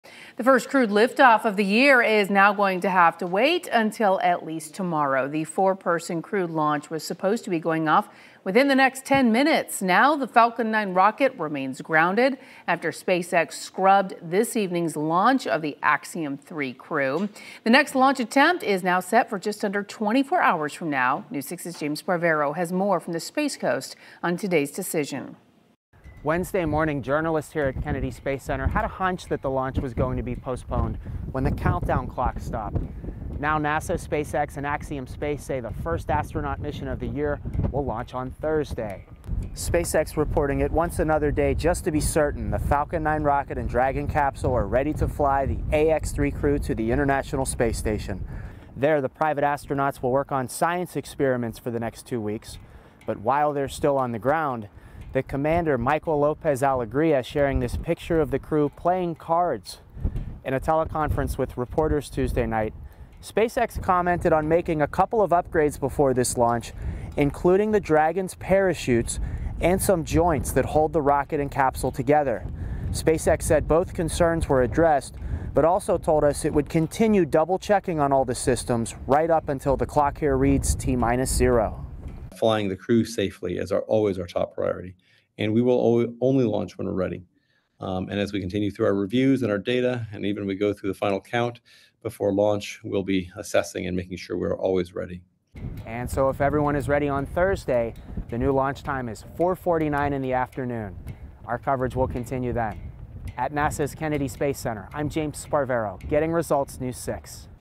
SpaceX scrubs Axiom-3 mission
SpaceX will now try again at 4:49 p.m. Thursday to launch the Axiom-3 crew to the International Space Station